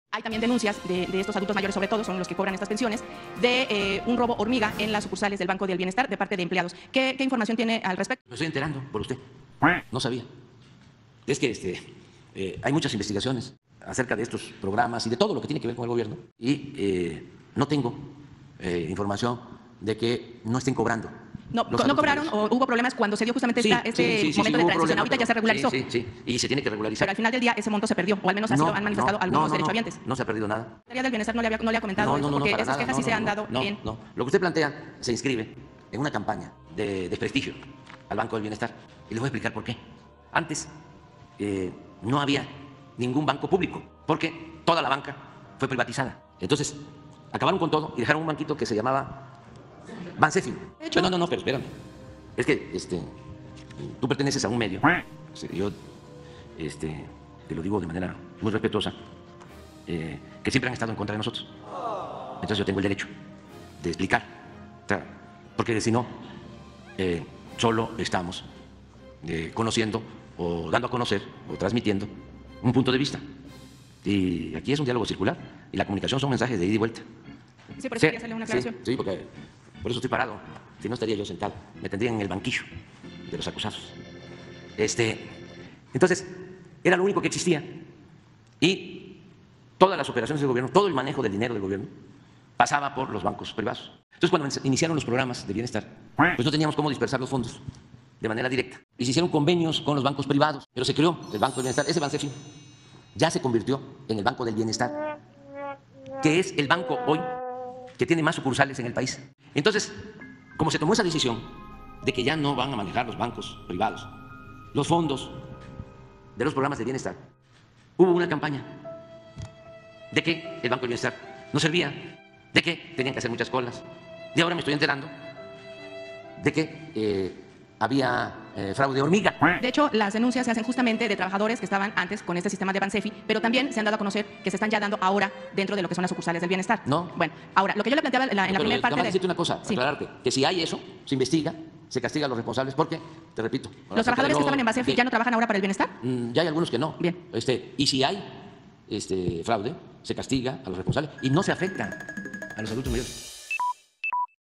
No hay robo hormiga en el banco del bienestar, todo es parte de una campaña de desprestigio 🦆
Con un desconocimiento total (dicho por él mismo), desacreditó las quejas de los cuentahabientes del Banco del Bienestar, que han sufrido un robo “hormiga” en sus pensiones de adultos mayores por parte de trabajadores de la institución bancaria, dice que es parte de una campaña para desacreditar todo lo que hace su gobierno 🦆